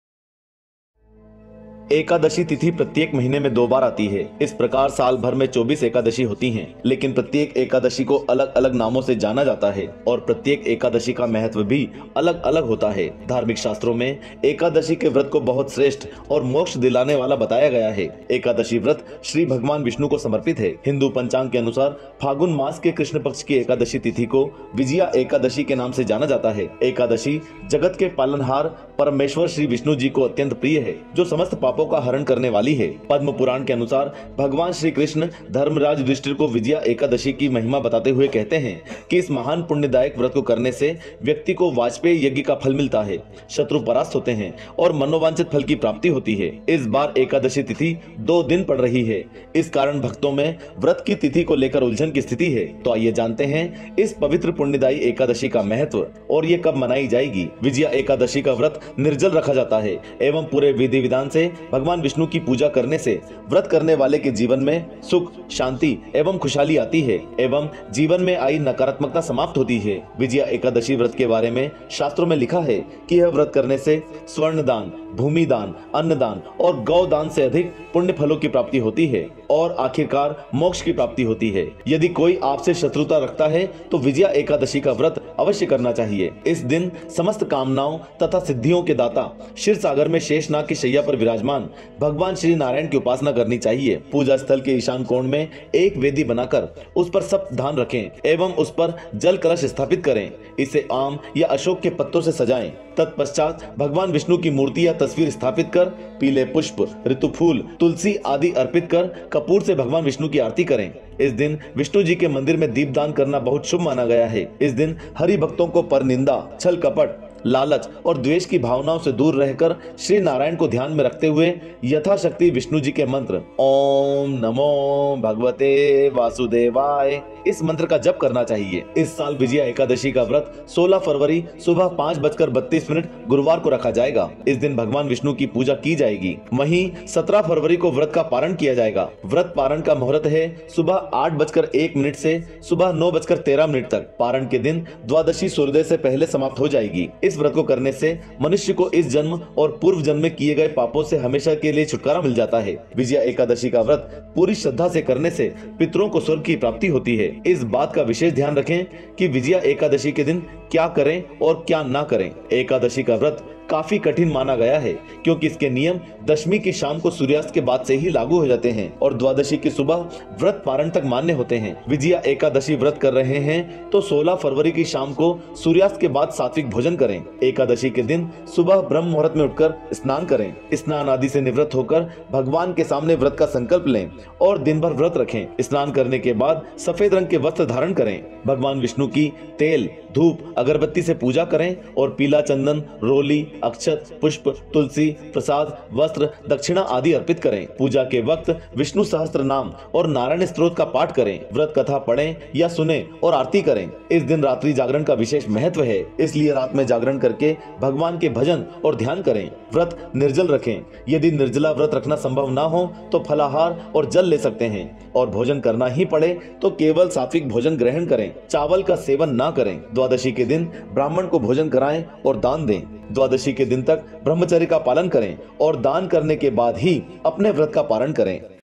विजया एकादशी का महत्व और शुभ मुहूर्त | Vijaya Ekadashi Vrat 2023.
Vedic Vox Presents:
Title:- विजया एकादशी का महत्व और शुभ मुहूर्त | Vijaya Ekadashi Vrat 2023.
Iss Video Me Hum Vijaya Ekadashi Ka Mahatva Aur Vijaya Ekadashi Ka Muhurt Janenge.